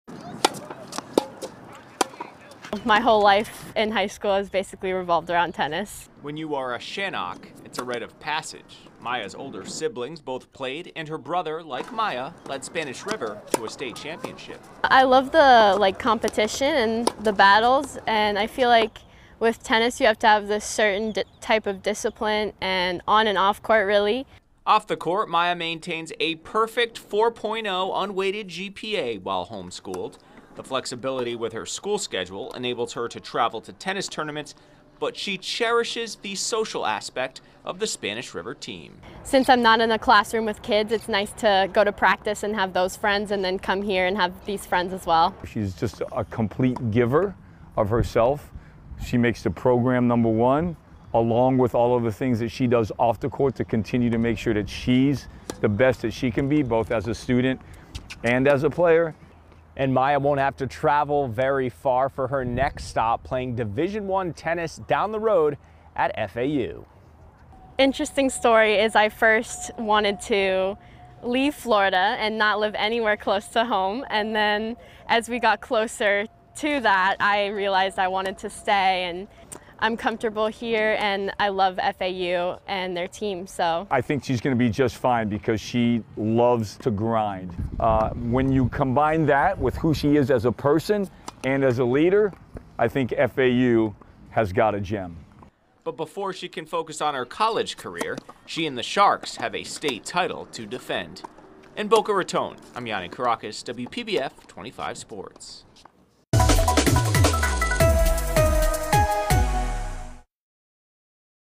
March Scholar Athlete of the Month: Maya Shanok
WPBF's Yianni Kourakis speak with our Scholar Athlete of the month: Maya Shanok. Shanok plays tennis at Spanish River Community High School. She committed to play Division 1 tennis at Florida Atlantic University next year.